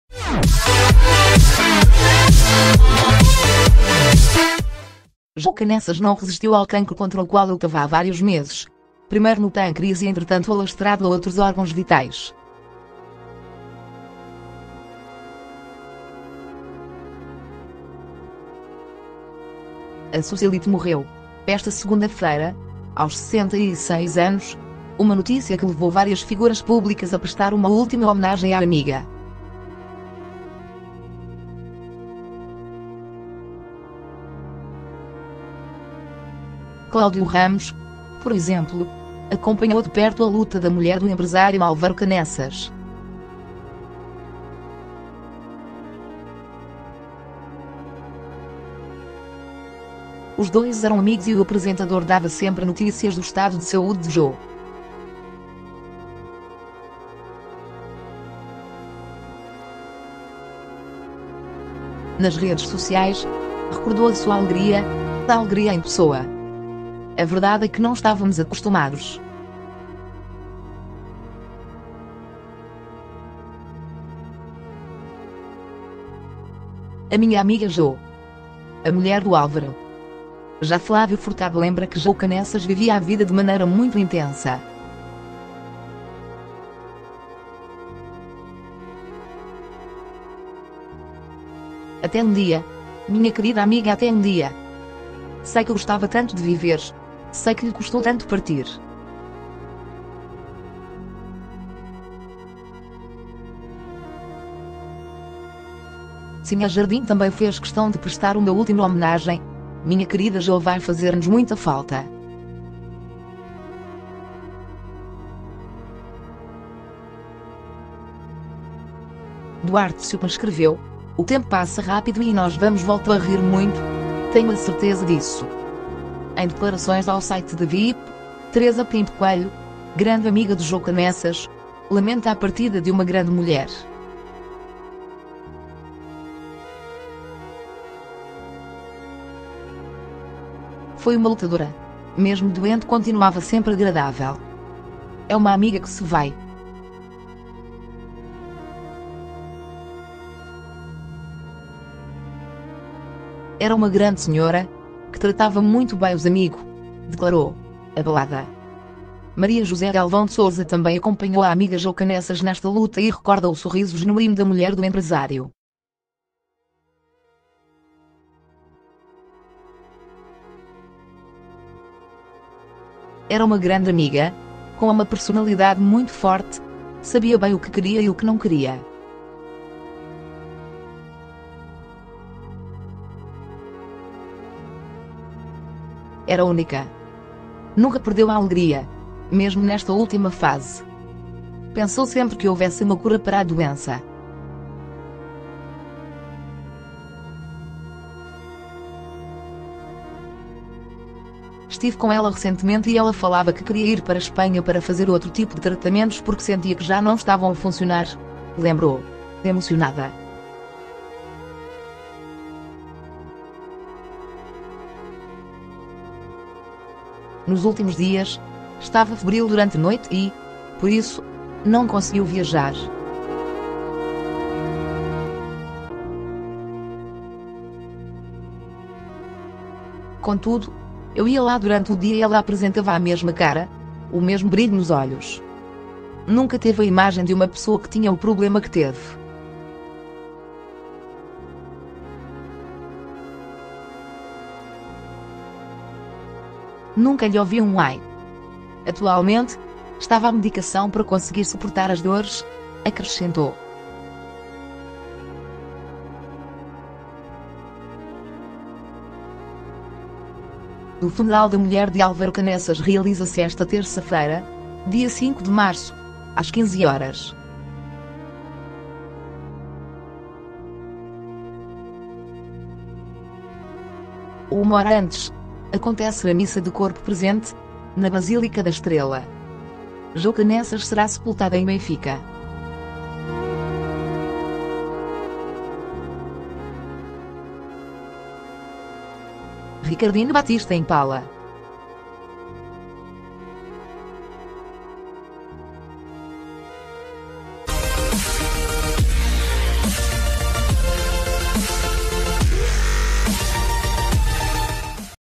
Amigos DESPEDEM-SE de Jô Caneças: «Estava febril e, por isso, não conseguiu viajar»
Jô Caneças não resistiu ao cancro contra o qual lutava há vários meses, primeiro no pâncreas e entretanto alastrado a outros órgãos vitais. A socialite morreu, esta segunda-feira, aos 66 anos, uma notícia que levou várias figuras públicas a prestar uma última homenagem à amiga.  Cláudio Ramos, por exemplo, acompanhou de perto a luta da mulher do empresário Álvaro Caneças. Os dois eram amigos e o apresentador dava sempre notícias do estado de saúde de Jô. A verdade a que não estávamos acostumados. A minha amiga Jô. A mulher do Álvaro.»  Já Flávio Furtado lembra que Jô Caneças vivia a vida de maneira muito intensa. «Até um dia, minha querida Amiga… até um dia. Sei que gostava tanto de viver, sei que lhe custou tanto partir.»  Cinha Jardim também fez questão de prestar uma última homenagem: «Minha querida Jô… vai fazer-nos muita falta».  Duarte Siopa escreveu: «O tempo passa rápido e nós vamos voltar a rir muito, tenho a certeza disso!»  Em declarações ao site da VIP, Teresa Pinto Coelho, grande amiga de Jô Caneças, lamenta a partida de «uma grande mulher». «Foi uma lutadora, mesmo doente continuava sempre agradável. É uma amiga que se vai. Era uma grande senhora, que tratava muito bem os amigo», declarou, abalada.   Maria José Galvão de Sousa também acompanhou a amiga Jô Caneças nesta luta e recorda o sorriso genuíno da mulher do empresário. «Era uma grande amiga, com uma personalidade muito forte, sabia bem o que queria e o que não queria. Era única. Nunca perdeu a alegria, mesmo nesta última fase. Pensou sempre que houvesse uma cura para a doença. Estive com ela recentemente e ela falava que queria ir para Espanha para fazer outro tipo de tratamentos porque sentia que já não estavam a funcionar», lembrou, emocionada.  «Nos últimos dias, estava febril durante noite e, por isso, não conseguiu viajar. Contudo, eu ia lá durante o dia e ela apresentava a mesma cara, o mesmo brilho nos olhos… Nunca teve a imagem de uma pessoa que tinha o problema que teve. Nunca lhe ouvi um ‘ai’. Atualmente, estava a medicação para conseguir suportar as dores», acrescentou.  O funeral da mulher de Álvaro Caneças realiza-se esta terça-feira, dia 5 de março, às 15 horas. Uma hora antes, acontece a missa de corpo presente, na Basílica da Estrela. Jô Caneças será sepultada em Benfica.    Ricardina Batista |  Impala